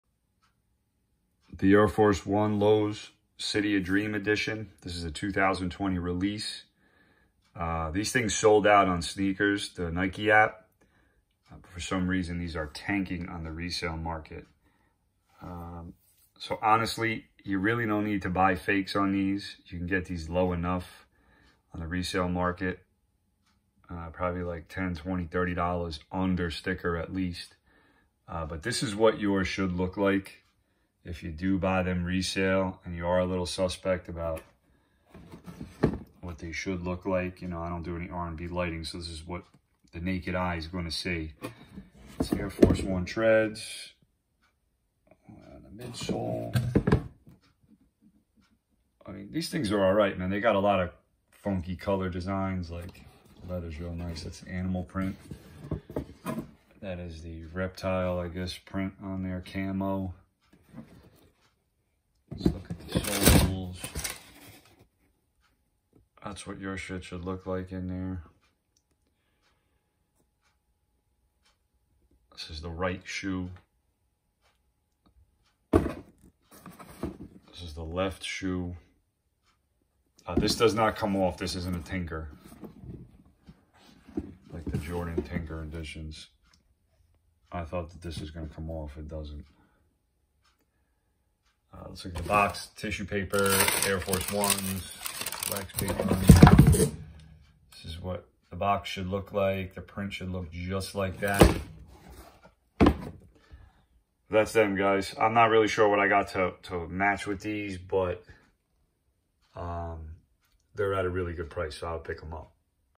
Air Force 1 City of Dreams 2020 unboxing review
Air Force 1 City of Dreams 2020 unboxing release from Nike.com